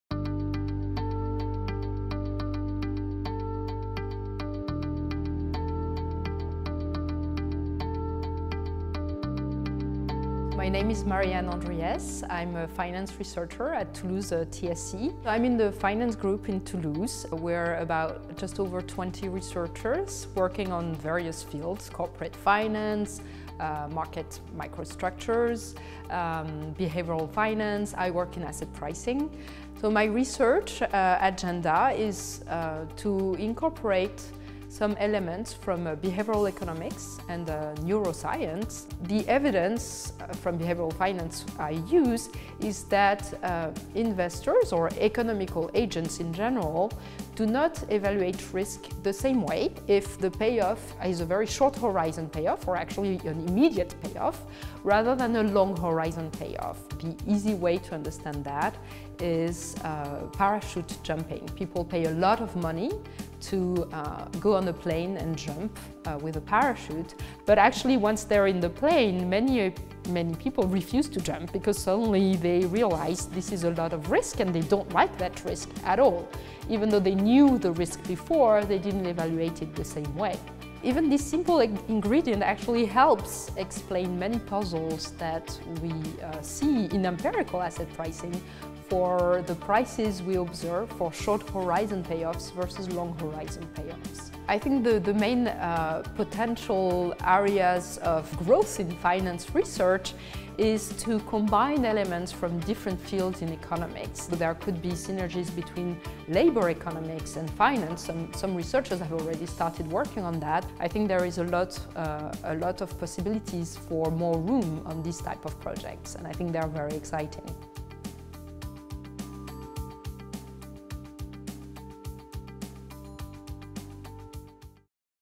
Inside Research - Marianne Andries - Finance
Marianne Andries is an Assistant Professor in Finance at TSE within the Finance research group. Her areas of interest are asset pricing and behavioral finance theory, using psychology to better understand financial phenomena.